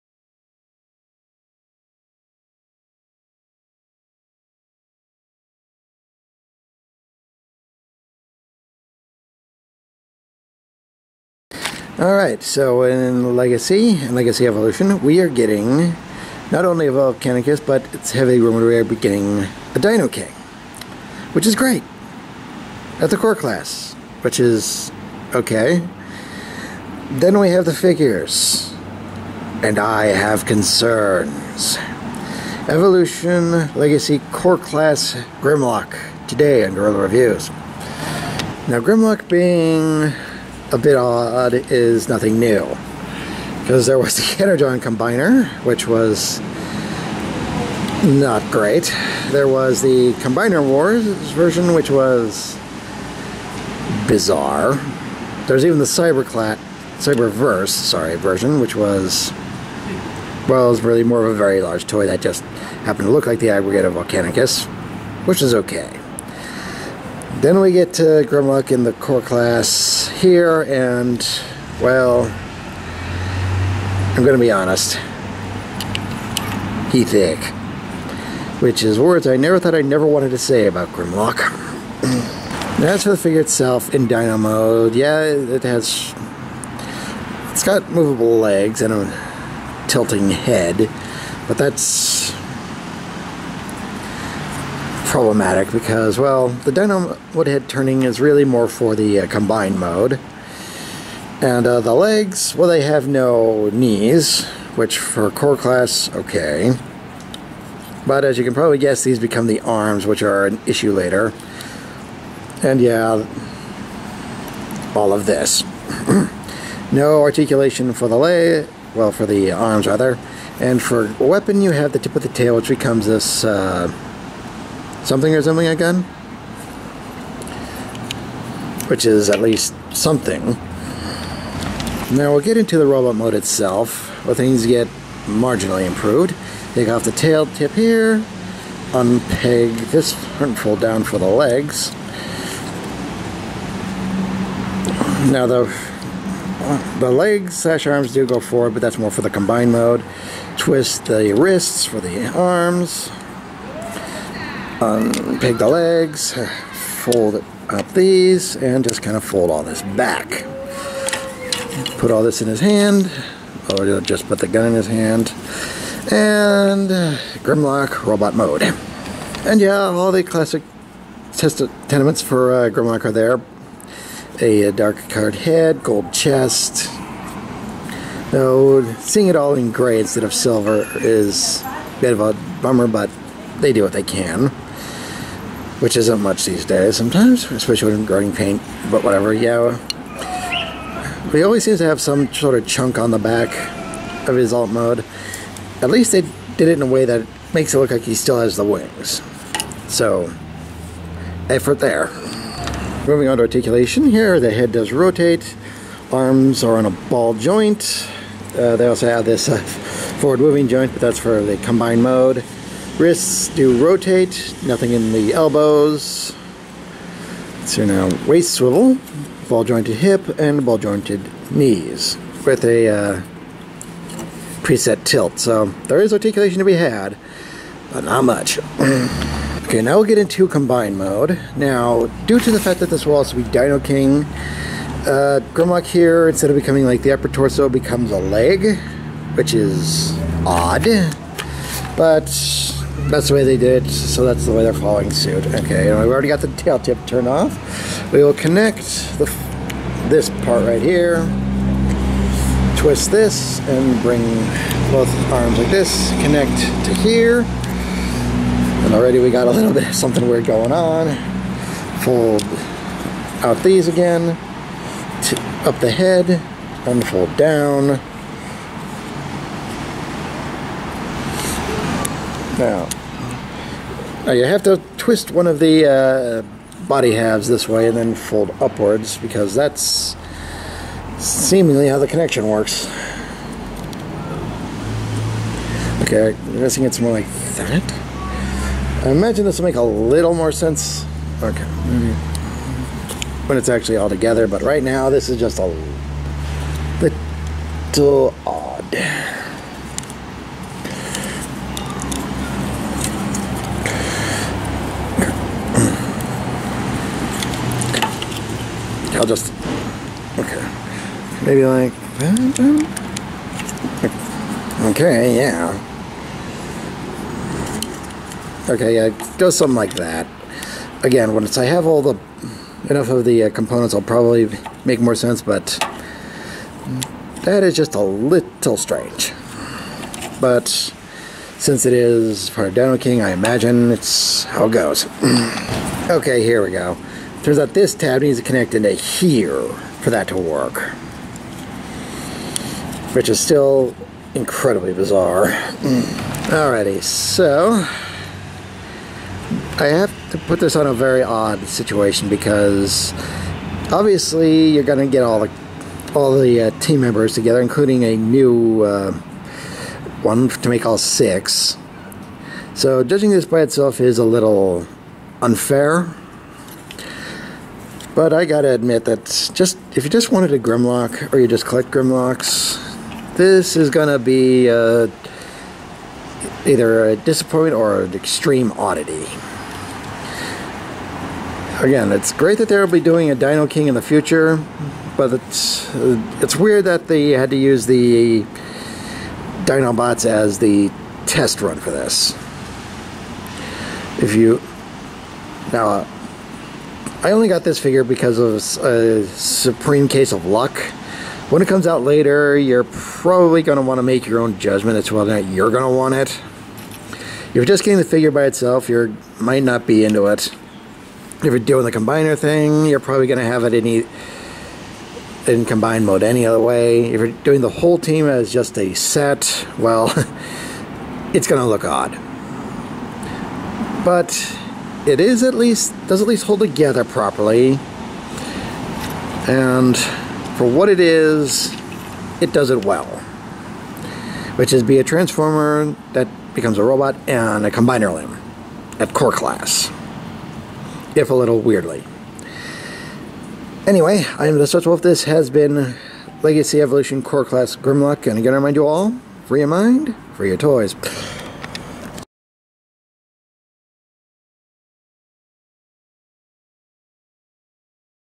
Guerrilla Reviews: Legacy Evolution Core class Grimlock
Luck is a funny thing. I managed to this, in a brick and mortar, able to buy it legitimately, and give my point of view on it. But is that a good thing? Let's Have a look.